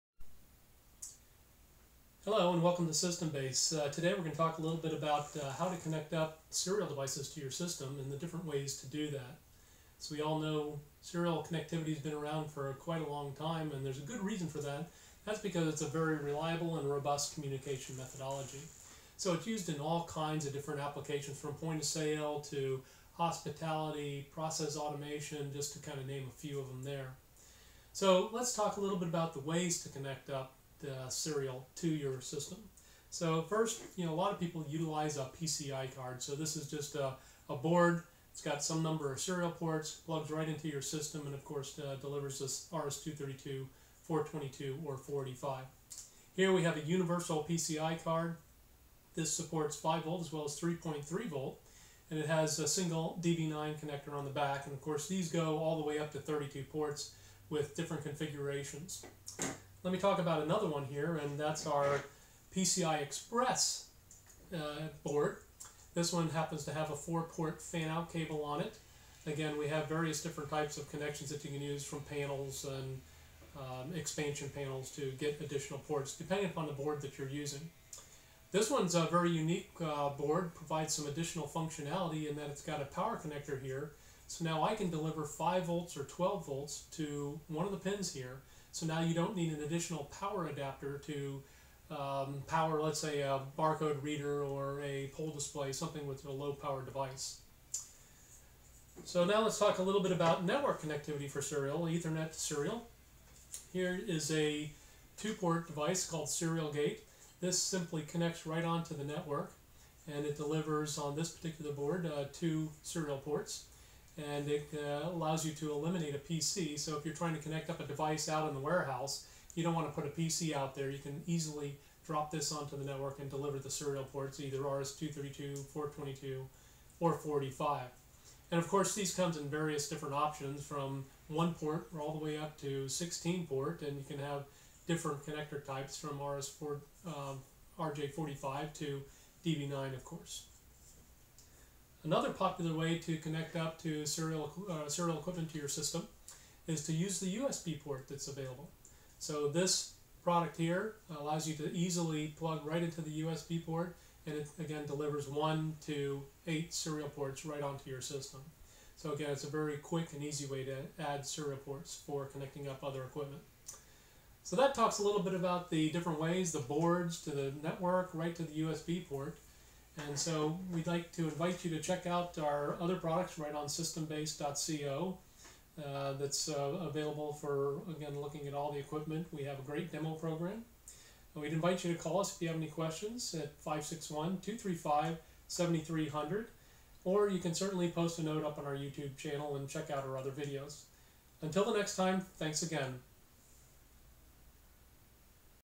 Serial Port Communication - Ways to Connect to Your System
Serial communication has been around for a long time because it's a proven technology. This video will explain some of the way to make a serial connection to your system. For more information you can contact SystemBase by visiting our website.